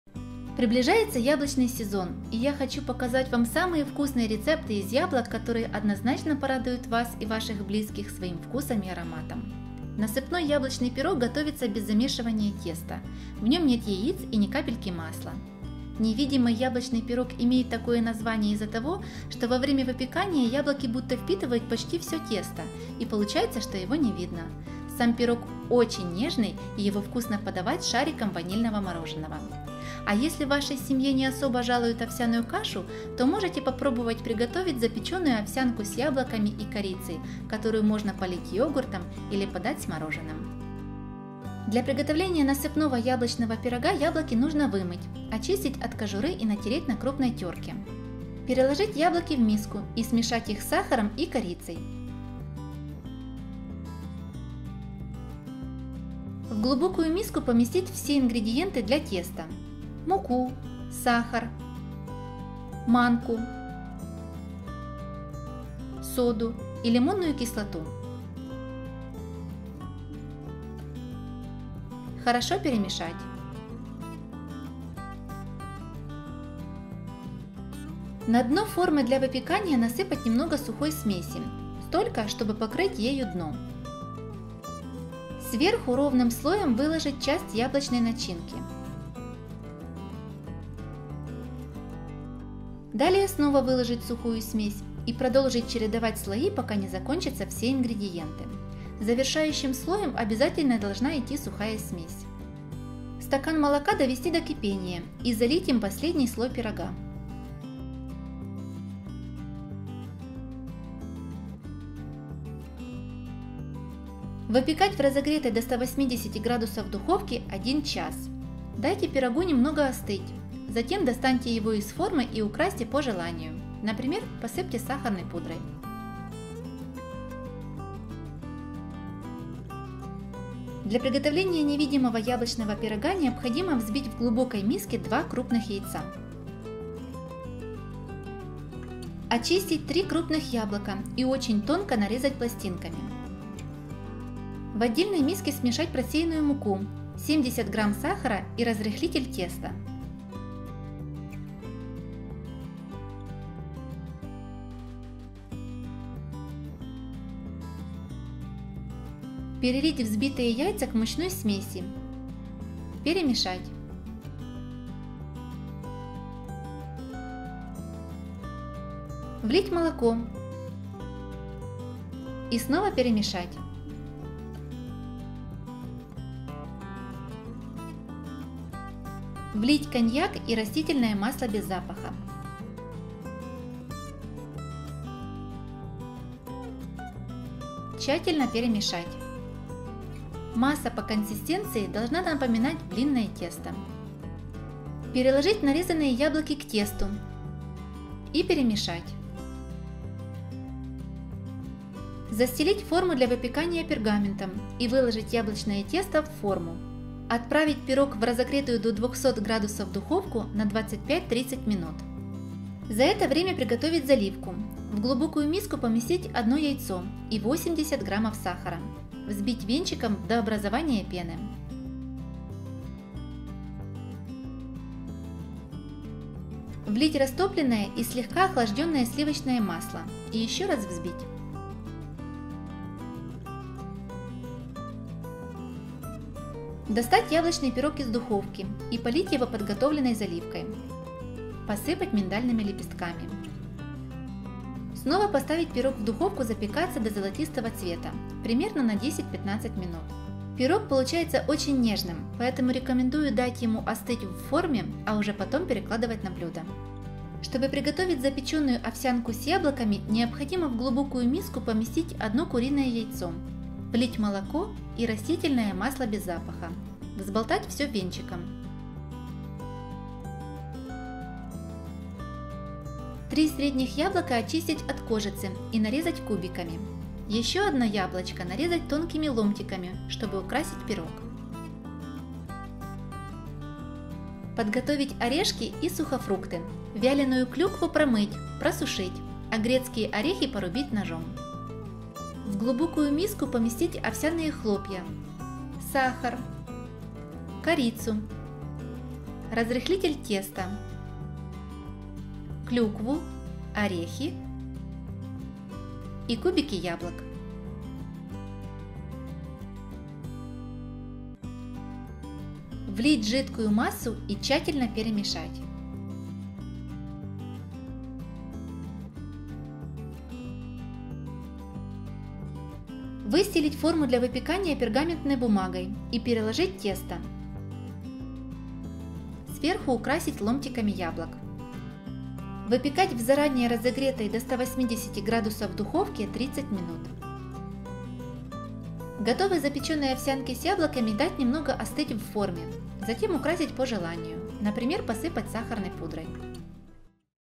3 СУПЕР-БЫСТРЫХ БЛЮДА ИЗ ЯБЛОК: НАСЫПНОЙ И НЕВИДИМЫЙ ПИРОГИ, ЗАПЕКАНКА
!Насыпной яблочный пирог - 0:41 мин.

Многим, наверное, знаком насыпной яблочный пирог, но для кого-то он станет настоящим открытием. Ведь для него не нужно замешивать тесто, в нем нет яиц и ни капельки масла. Я обязательно пеку его в яблочный сезон, это отличная альтернатива шарлотке, но без яиц. В качестве начинки обычно использую яблоки, а когда начинается сезон тыквы, то тыкву со специями.

Ингредиенты:

Сухая смесь
Мука – 1 стак.
Сода – 1 ч. л.
Крупа манная – 1 стак.
Кислота лимонная – 1 щепотка
Сахар – 1 стак.

Начинка:

Яблоко – 1 кг (или чуть больше)
Молоко – 1 стак.
Сахар – 1/2 стак. (больше или меньше в зависимости от того, насколько сладкие яблоки)
Корица – 1 ч. л. (молотая)
Сахарная пудра – 1 ст. л. (для подачи)
Масло растительное – для смазывания формы

Приготовление:

- Поставьте разогреваться духовку до 180 гр. Смажьте растительным маслом форму 18 на 18 см для выпечки.

- Смешайте сахар, муку, манку, соду и лимонную кислоту. Отставьте в сторону.

- Удалите сердцевину и натрите яблоки на крупной терке. Очень выручает кухонный комбайн. Смешайте яблоки с корицей и сахаром. Необходимое количество сахара регулируйте по своему вкусу.

- На дно формы насыпьте немного сухой смеси (столько, чтобы прикрыть ею дно). Сверху ровным слоем выложите часть яблок. Повторите, пока не кончатся все ингредиенты. Завершающим слоем должна идти сухая смесь. Я обычно делаю 5 слоев смеси для теста и между ними 4 яблочных слоя.

Выпекайте в разогретой духовке 1 час. Дайте пирогу немного остыть. Затем проведите ножом между краем пирога и формой и переверните его на блюдо. Перед подачей посыпьте пирог сахарной пудрой.

!«Невидимый» яблочный пирог - 2:10 мин.

Интересное название у пирога, не так ли? Именно из-за того, что во время выпечки яблоки будто впитывают почти все тесто, оно и получается невидимым.

Ингредиенты:

Яблоко — 3 шт. (крупные яблоки)
Сахар — 150 г (70 г - тесто, 80 г - заливка)
Яйцо куриное — 3 шт. (2 шт. - тесто, 1 шт. - заливка)
Разрыхлитель теста — 15 г
Молоко — 90 мл
Коньяк — 1 ст.л.
Масло растительное — 2 ст.л.
Масло сливочное — 2 ст.л.
Миндальные лепестки — 3 ст.л.

Приготовление:

- В миске смешать просеянную муку, 70 г. сахара и разрыхлитель.

- Отдельно взбить 2 яйца в пенистую массу.

- Перелить взбитые яйца к мучной смеси. Перемешать.

- Влить молоко. Перемешать. На этом этапе тесто должно пениться и чуть увеличиться в размере.

- Влить растительное масло и коньяк.

Перемешать еще раз тесто.

- Очистить 3 крупных яблока и тонко, очень тонко нарезать пластинками. Удобно воспользоваться специальной теркой-шинковкой.

- Пластинки должны быть тоненькими. Это очень важно и эффект невидимого теста будет достигнут только при такой тонкой нарезке яблок.

- Переложить пластинки яблок к тесту и перемешать. В миске будут одни яблоки, а самого теста практически не будет заметно. Так и должно быть.

Застелить форму пергаментом. Желательно брать форму небольшого размера, например d 18 см, тогда и пирог будет выше.

Вылить яблочное тесто в форму. Чуть разровнять яблоки сверху.

Хорошо прогрев духовку до 200 гр., отправить туда пирог на 25-30 минут.

- Венчиком взбить 1 яйцо и 80 г сахара до образования пены, влить растопленное и слегка охлажденное сливочное масло.

- Достать яблочный пирог из духовки, вылить заливку.

- Посыпать миндальными лепестками. Снова поставить пирог в духовку при 200 гр. на 15-15 мин.

!Запеченная овсянка с яблоком и корицей - 4:17 мин.

Если овсяную кашу не особо жалуют в Вашей семье, можно попробовать приготовить вкусную и ароматную запеканку-пирог – запеченную овсянку с яблоками и корицей. Украсьте десерт ягодами, полейте йогуртом или положите рядом шарик мороженого.

Ингредиенты:

Яблоко — 3 шт. (+1 для украшения)
Хлопья овсяные — 175 г
Сахар — 80 г
Разрыхлитель теста — 1.5 ч.л.
Орехи — 30 г
Клюква вяленая — 30 г

Приготовление:

- Включить духовку разогреваться до 180 гр.

Нужно взять крупные овсяные хлопья не быстрого приготовления. Желательно брать такие, чтобы на упаковке было указано время приготовления от 10 минут.

- 3 средних яблока очистить от кожицы и нарезать кубиками.

- Подготовить орешки и сухофрукты. Подойдут любые.

- В отдельной миске смешать яблочные кубики, крупные овсяные хлопья, сахар, разрыхлитель, корицу и орехи с сухофруктами.

- Отдельно вилкой или ручным венчиком взболтать крупное яйцо, молоко и растительное масло.

- К сухим ингредиентам влить жидкую часть и перемешать лопаткой.

- Выстелить небольшую форму для выпечки пергаментной бумагой или смазать ее растительным маслом и перелить достаточно густое комковатое тесто. Сверху украсить кусочками яблок.

- Выпекать овсянку с яблоками 30 мин, запеканка должна хорошо пропечься и зарумяниться.